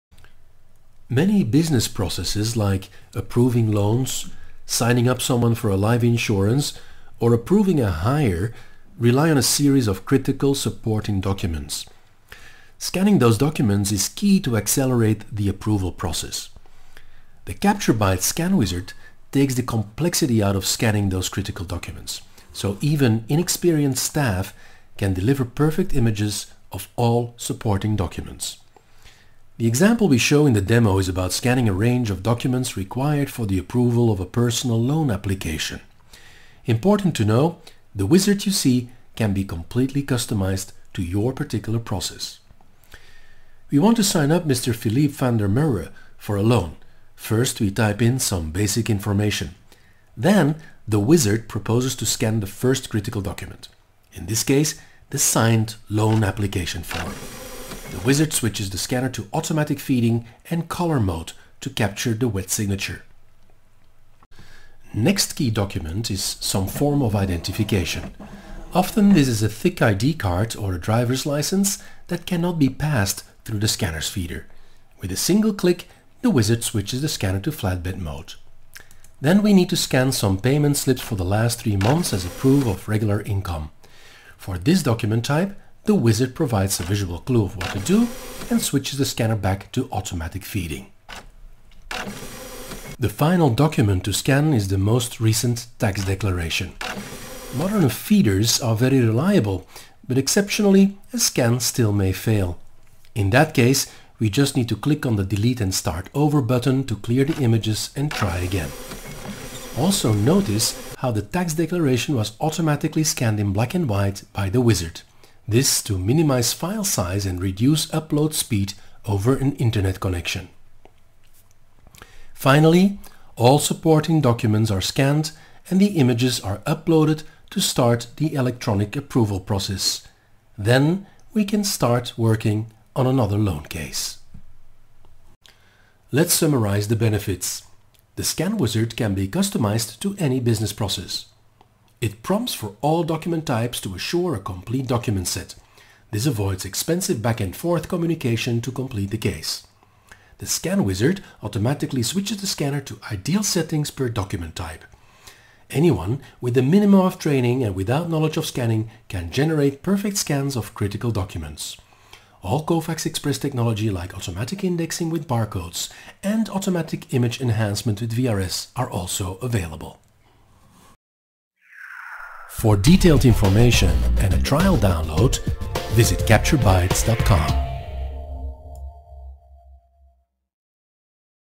CaptureBites™ Commander - Scan Wizard
Many business processes, like approving loans, signing up someone for a life insurance or approving a hire, rely on a series of critical supporting documents.  Scanning those documents is key to accelerate the approval process.

Building a scan wizard is an application of the CaptureBites Commander Tool. It takes the complexity out of scanning those critical documents. So even inexperienced staff can deliver perfect images of all supporting documents.

The example we show in the movie is about scanning a range of documents required for the approval of a personal loan application, but the wizard can be completely customized to your particular process.